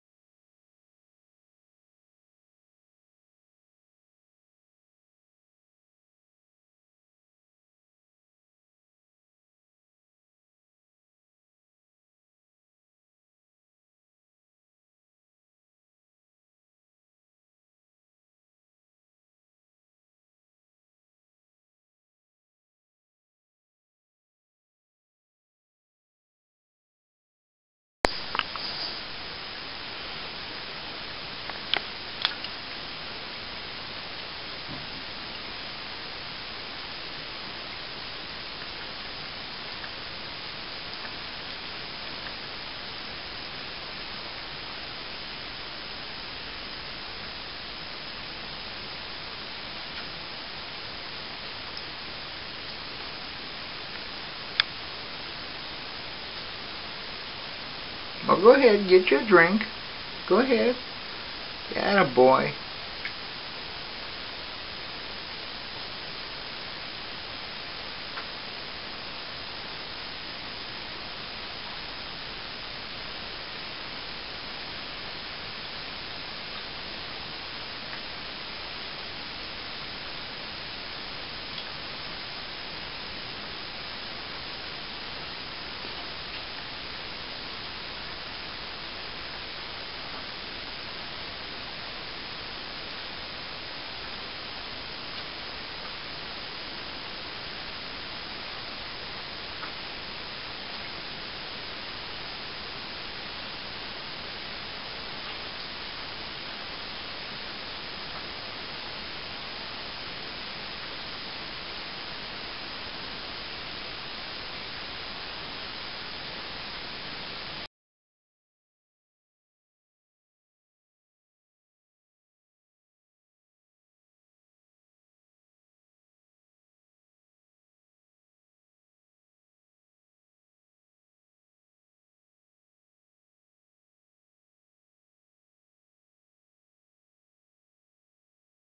Cat "Howler" Bellies Up To The Bar
Cat "Howler" won't drink from a water dish... He has to have his own private fountain